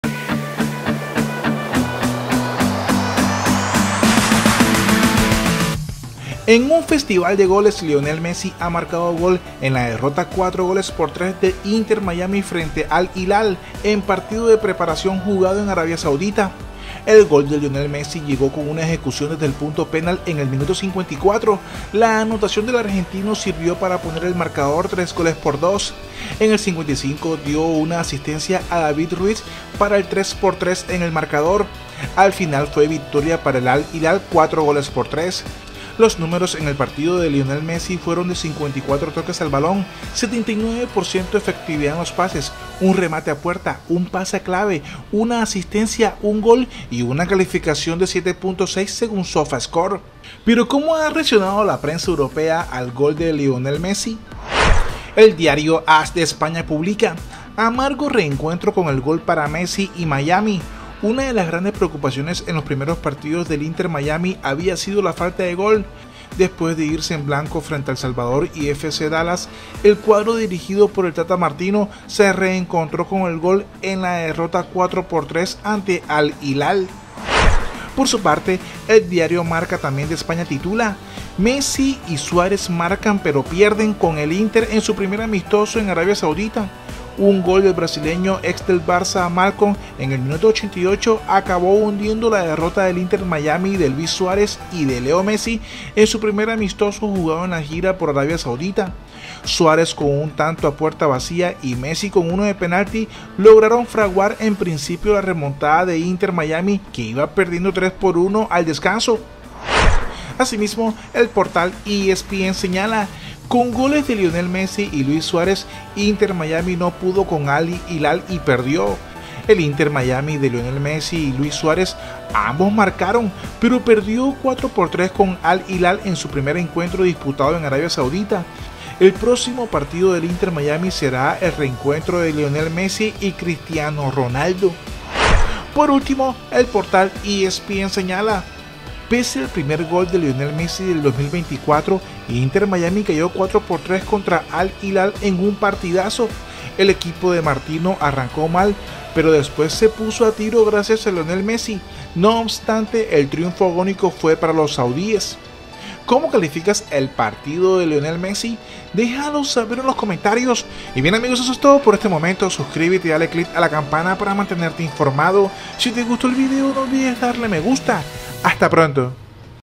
ASI REACCIONA PRENSA EUROPEA a GOL de MESSI INTER MIAMI vs AL HILAL
En festival de goles Lionel Messi ha marcado gol en la derrota 4-3 de Inter Miami frente a Al Hilal en partido de preparación jugado en Arabia Saudita.
El gol de Lionel Messi llegó con un ejecución desde el punto penal en el minuto 54. La anotación del argentino sirvió para poner el marcador 3-2.
En el 55 dio a la asistencia a David Ruiz para el 3-3 en el marcador.
Al final fue victoria para Al Hilal 4-3.
Los números en el partido de Lionel Messi fueron de 54 toques al balón, 79 % de efectividad en los pases, un remate a puerta, un pase clave, una asistencia, un gol y un calificación de 7.6 según Sofascore.
Pero cómo ha reaccionado la prensa inglesa al gol de Lionel Messi.